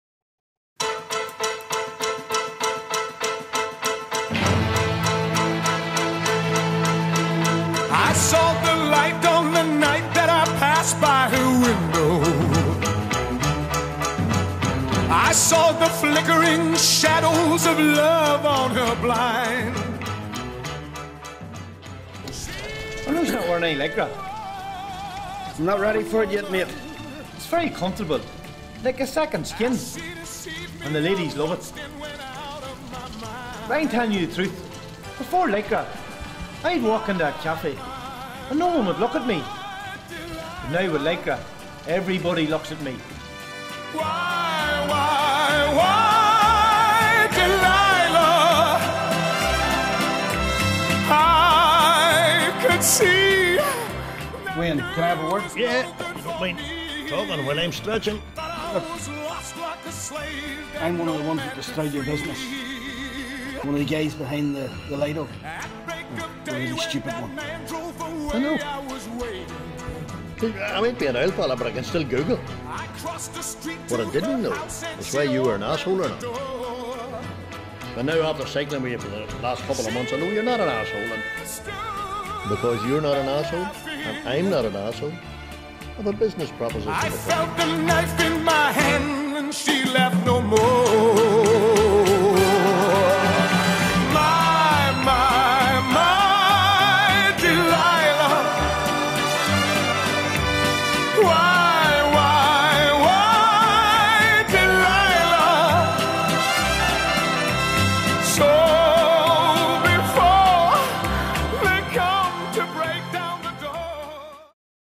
MAMIL @ Craic Theatre
MAMIL @ the Craic Theatre Coalisland April 5th-8th & 12th - 15th

MAMIL is an hilarious comedy about men and mid life crisis. Bryan Cook is a property developer who has accumulated a small fortune building leaky homes. He’s a stressed out, self-medicating, self-loathing arse, killing himself to increase his bank balance while decreasing his golf score. When his business and life fall apart, he joins a men’s cycling group to relieve the stress and get healthy…